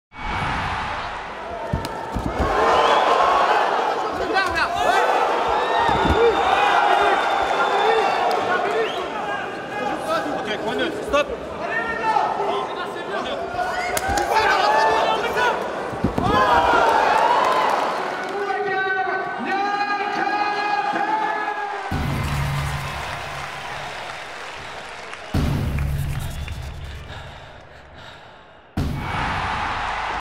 100% Fight 41 | Bonus: Niakate vs Oudgou
Le double Champion des Contenders Boubakar Niakaté fera son grand retour le samedi 19 octobre prochain sur la carte du 100%FIGHT 41. En attendant, retrouvez les meilleurs moments de son combat face à Reda Oudgou lors du 100%FIGHT 24.